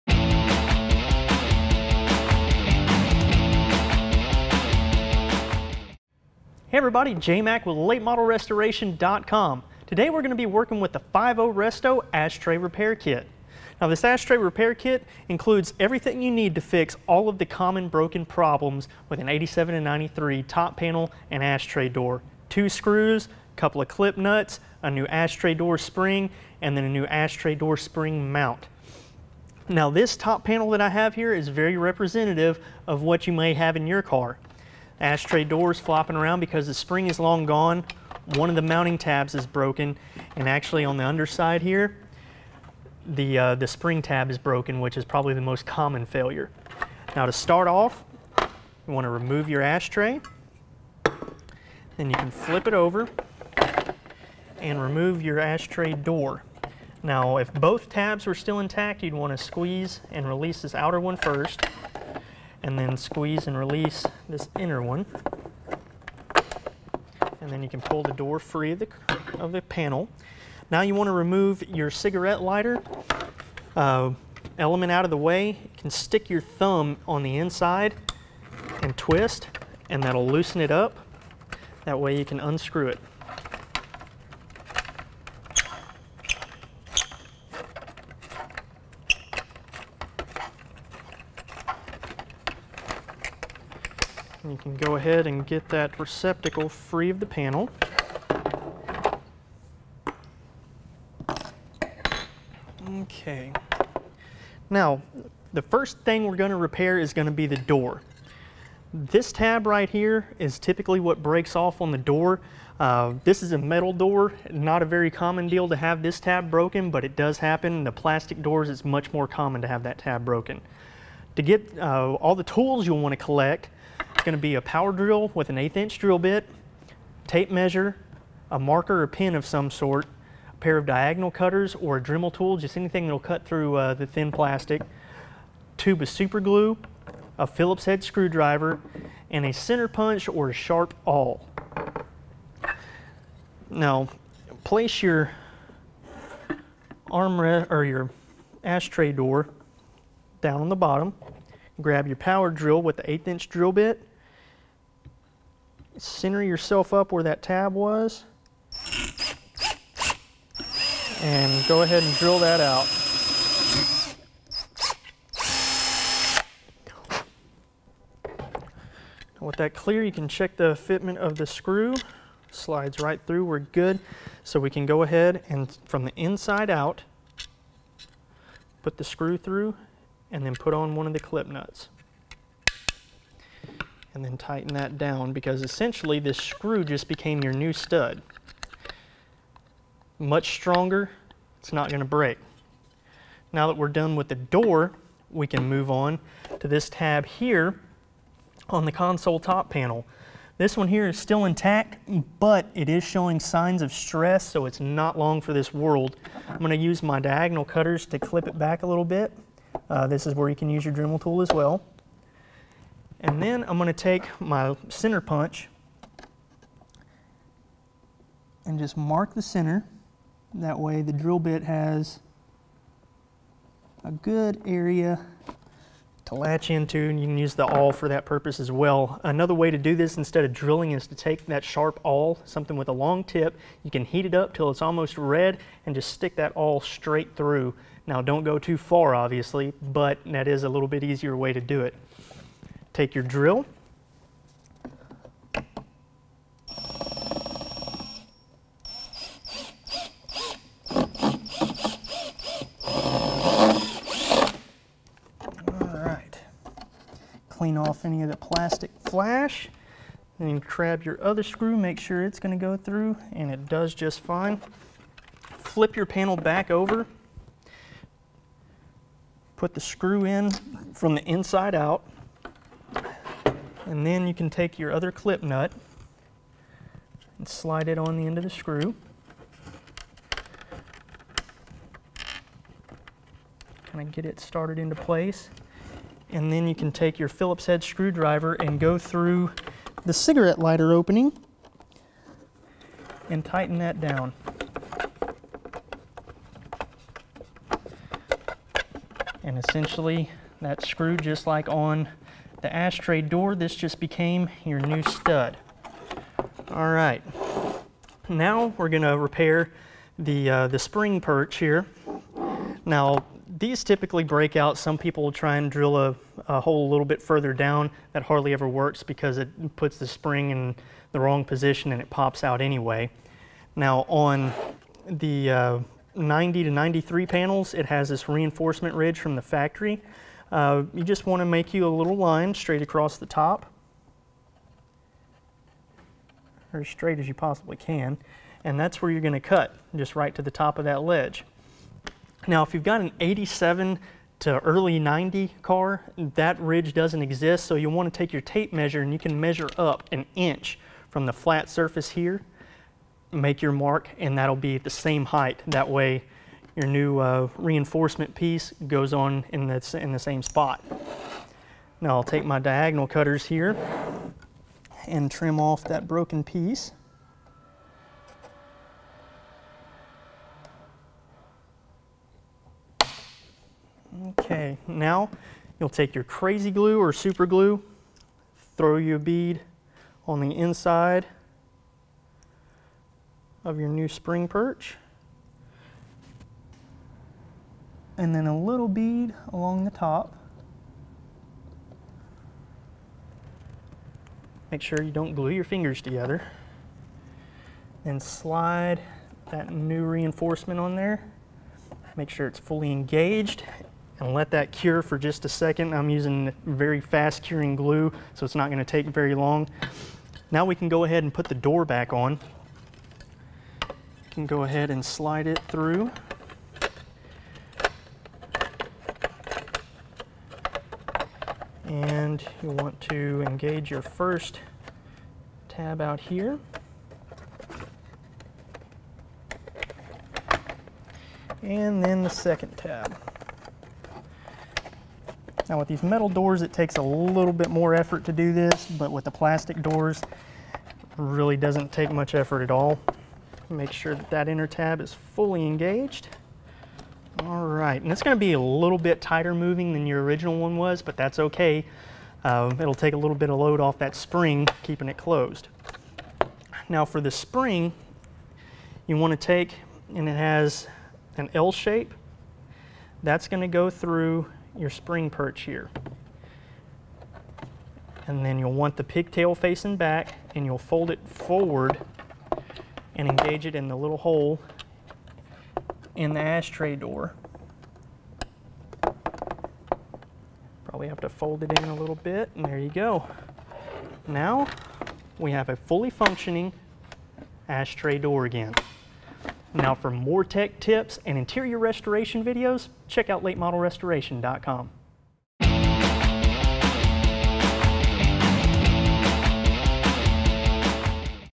1987-1993 Fox Body Mustang: Ash Tray Lid Repair Kit Installation
This ash tray repair kit is now sold under the DCR (Daniel Carpenter Reproductions) brand.

If you've been around Fox Body Mustangs long enough then you know the ash tray door is a common failure item in the interior.  Follow along in the video as Jmac shows you how to repair your broken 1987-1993 Fox Body Mustang ash tray lid.